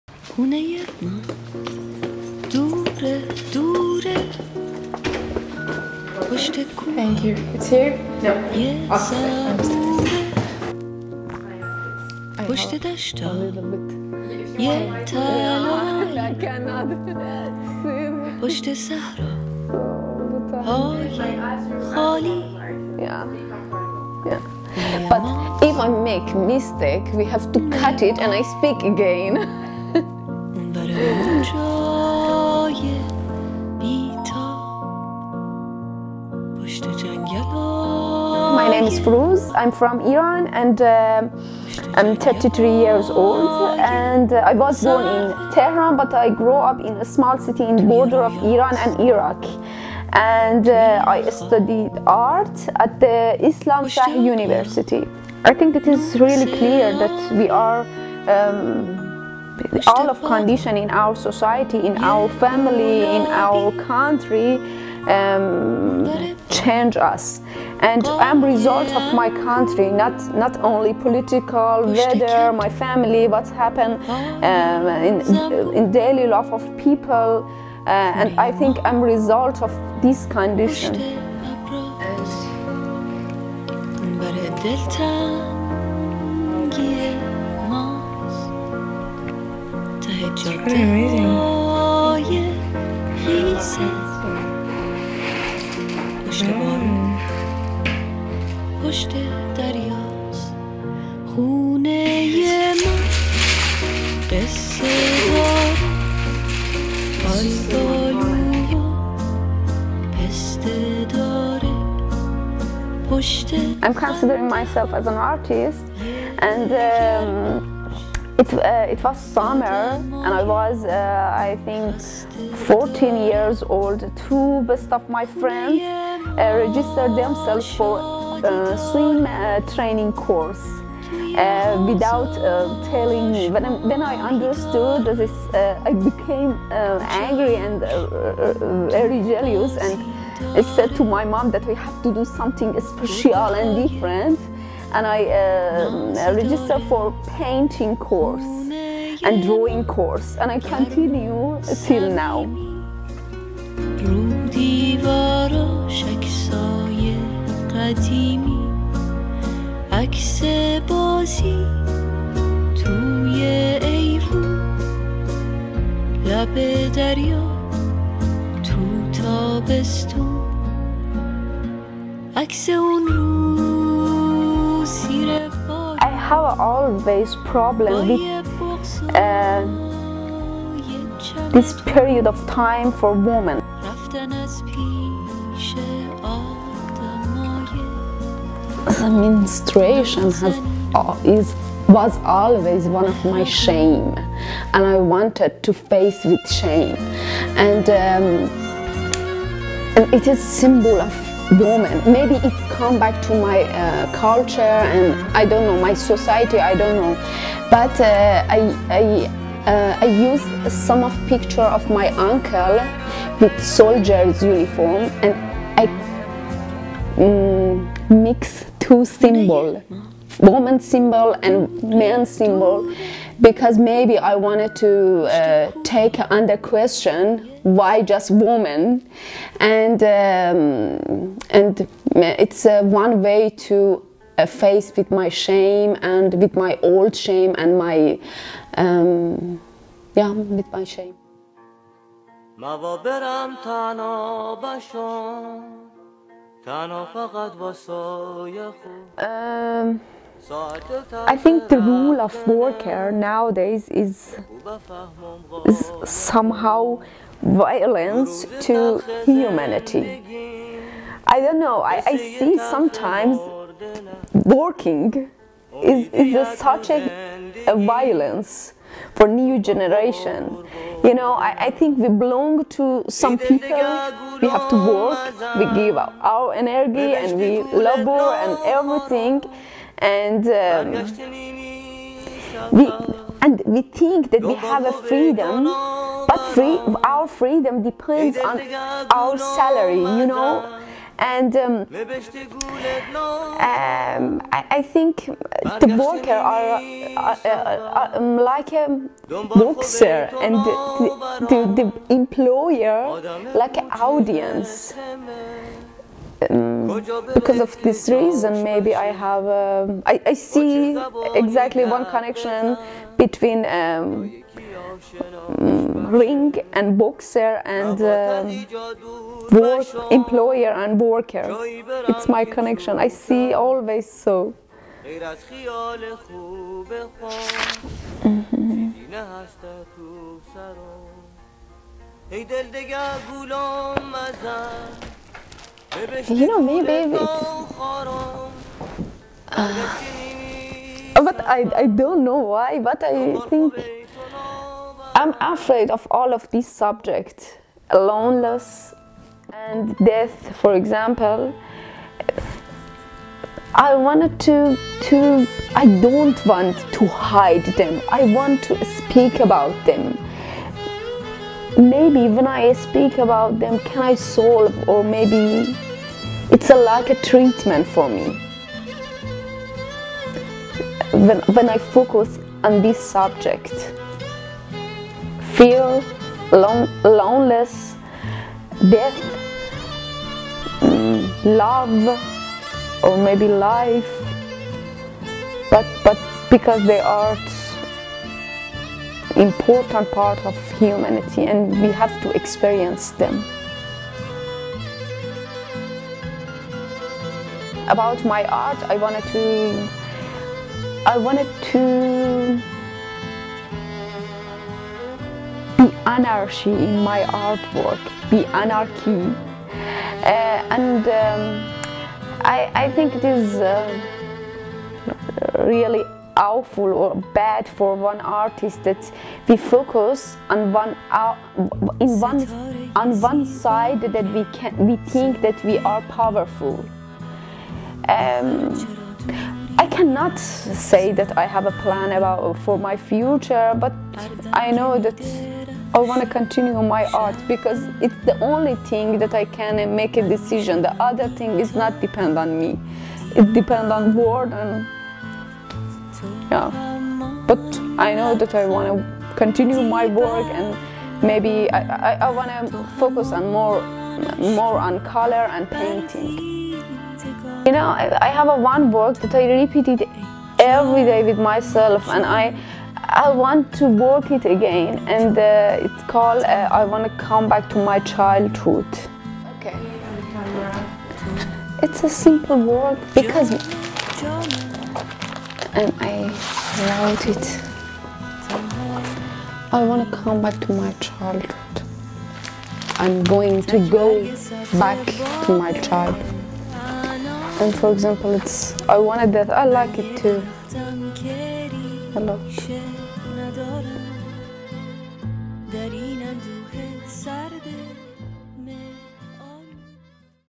Meet with Forouz Zarei - Iranian artist living in Berlin (SUB FR)
Forouz Zarei is an Iranian artist who lives in Berlin. She does drawing and painting in an terrific way. I fall in love with her work, that's why I'd like to share with you this little video. She told me more about her inspirations, the themes of some of her work and her future plan.

Forouz Zarei est une artiste berlinoise d'origine iranienne. Elle peint et dessine des oeuvres avec un talent hors du commun. Je suis tombée amoureuse de son travail et j'ai donc décidé de vous en faire une petite vidéo. Elle me parle de ses inspiration, des thèmes abordés dans son travail, de ses futurs projets, etc.